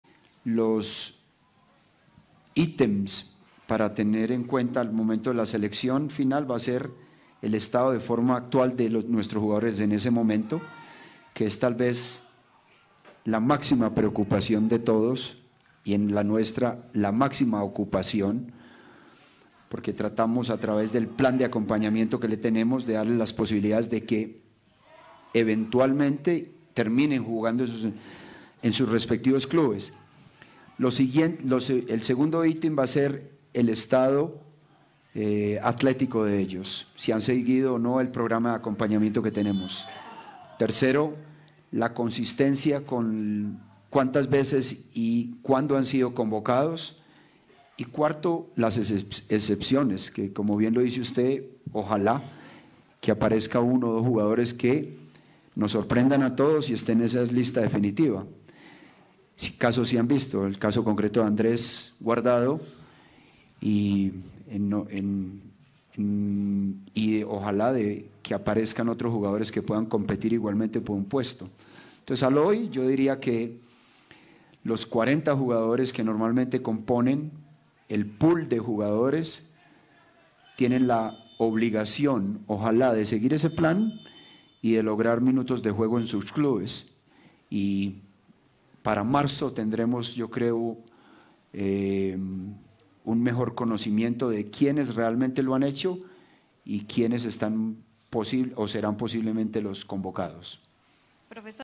¿Cuándo tendrá Osorio lista final del Tri para Rusia 2018? Respondió
Mira lo que contestó el técnico de la Selección Mexicana, sobre los 40 jugadores que siempre tiene pensados